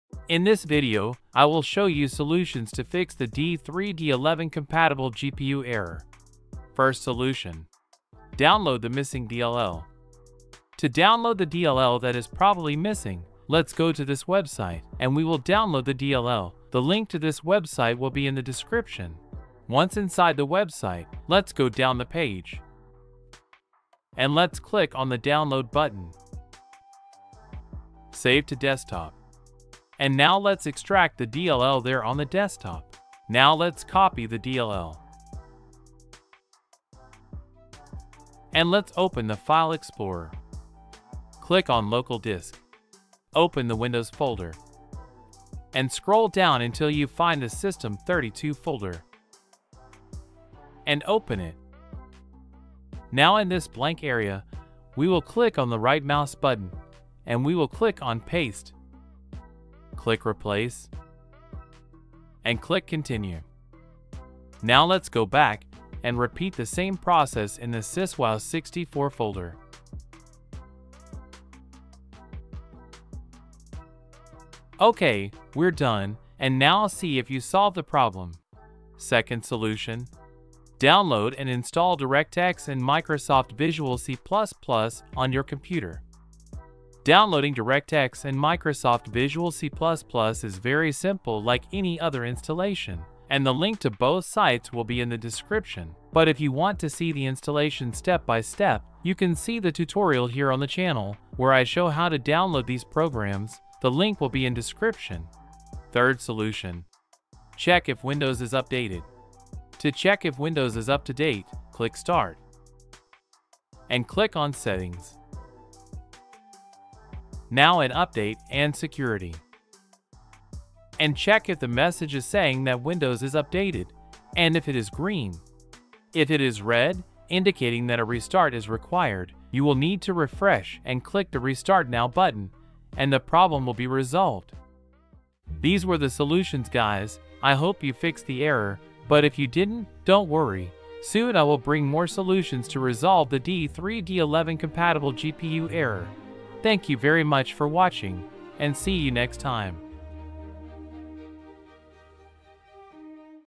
How To Fix A D3D11-Compatible GPU Error (3 SOLUTIONS)
► In this video I show you solutions to fix the D3D11 compatible GPU error in 2023!

0:00 Introduction
0:06 First Solution
1:19 Second Solution
1:46 Third Solution
2:15 End.